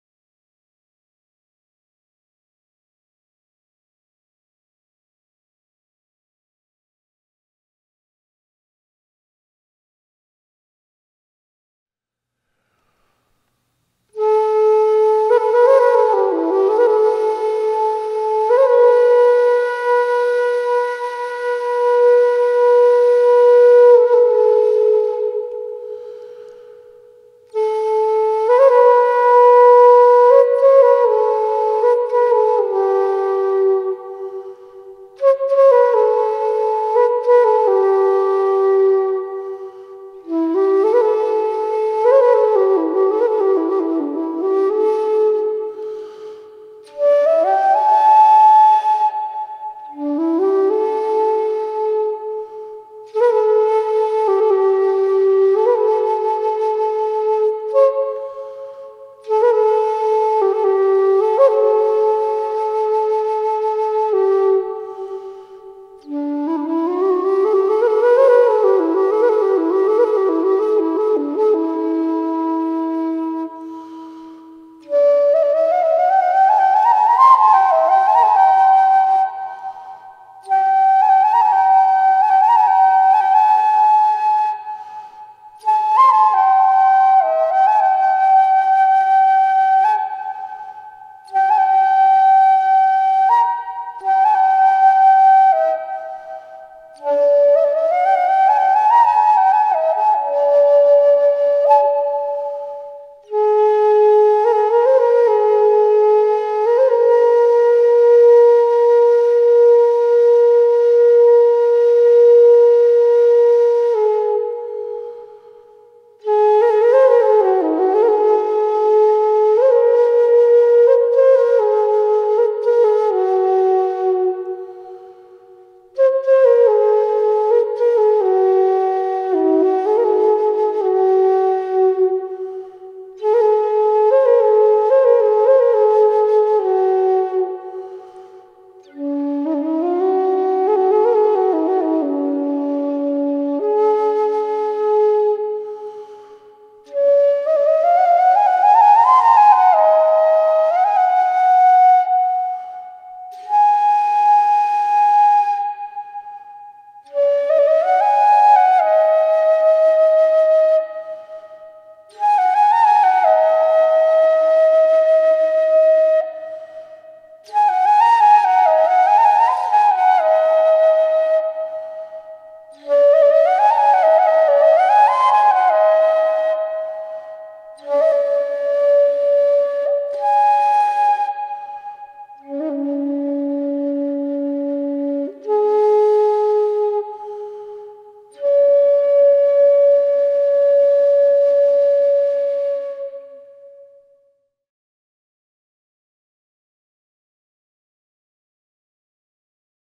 Two D Major Bamboo Flutes by Romy Benton
Here are two from my collection of Romy Benton Bamboo flutes. I recorded them using an AKG C1000s Microphone with an Electro Harmonix Holy Grail effects pedal inserted into an M-Audio Fast Track Pro interface.